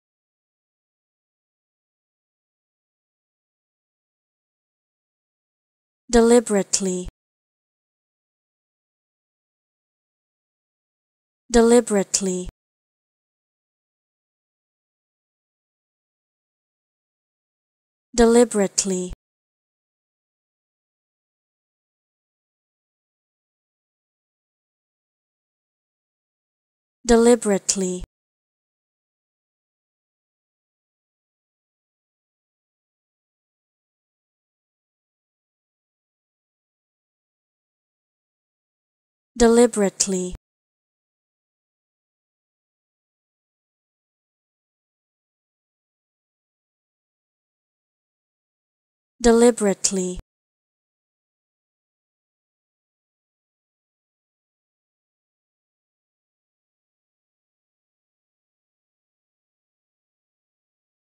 How To Say Deliberately
Learn how to say Deliberately with EmmaSaying free pronunciation tutorials.